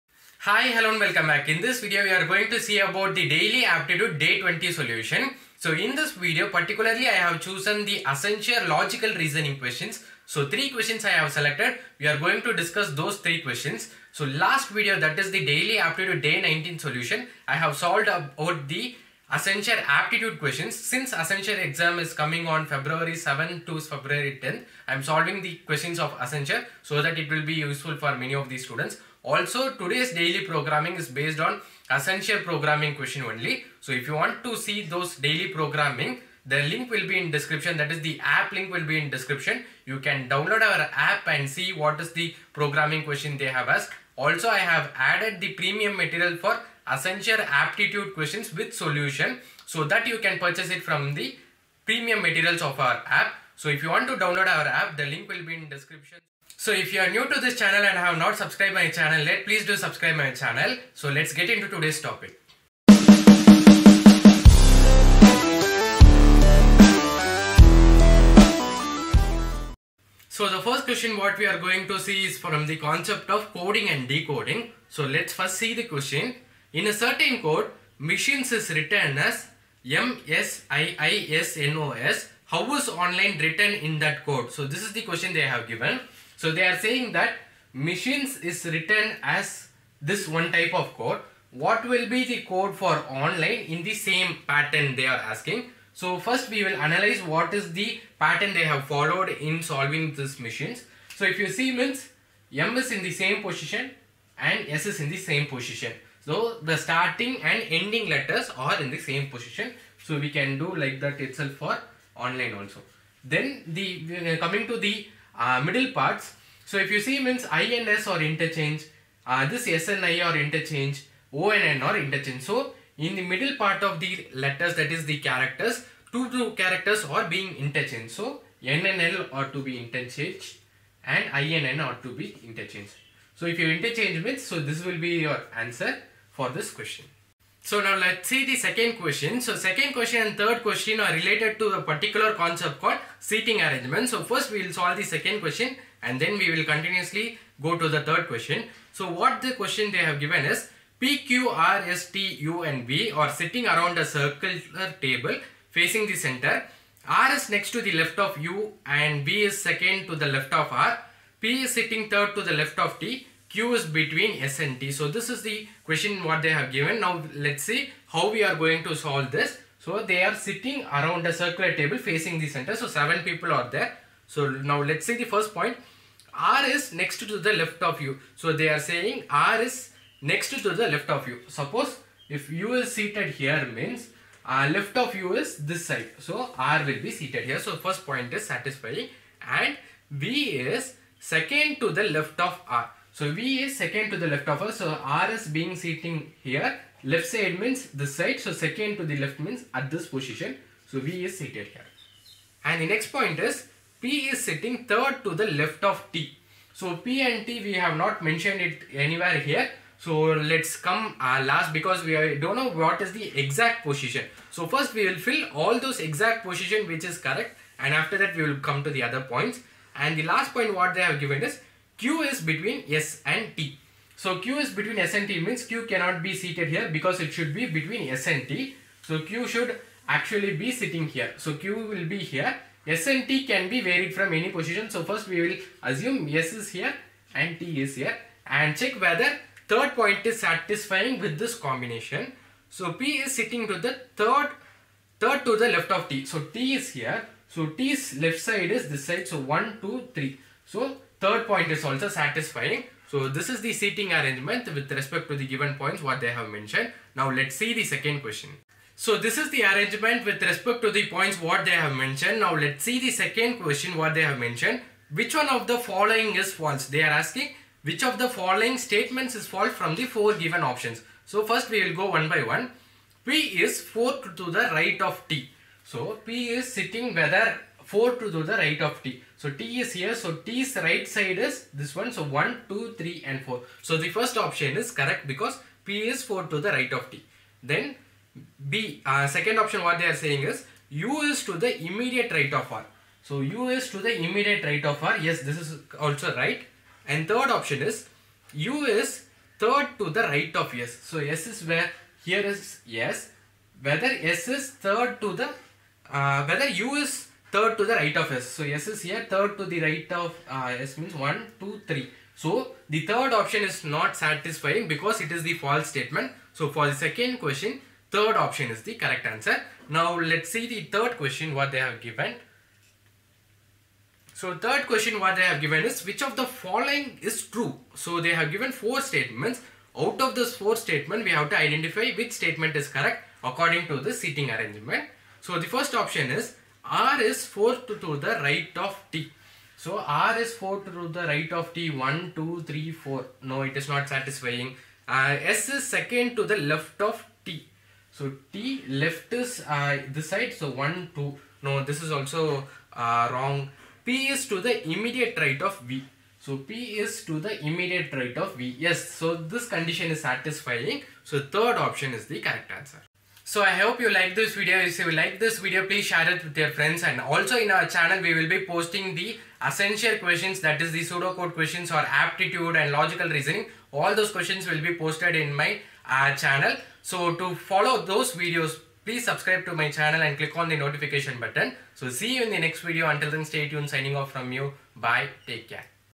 Accenture Logical Reasoning | Daily Aptitude - Day 20 | BiNaRiEs
In this video, I will be discussing with you Daily Aptitude - Day 20 Solution which was posted on 06/02/2022 on our APP.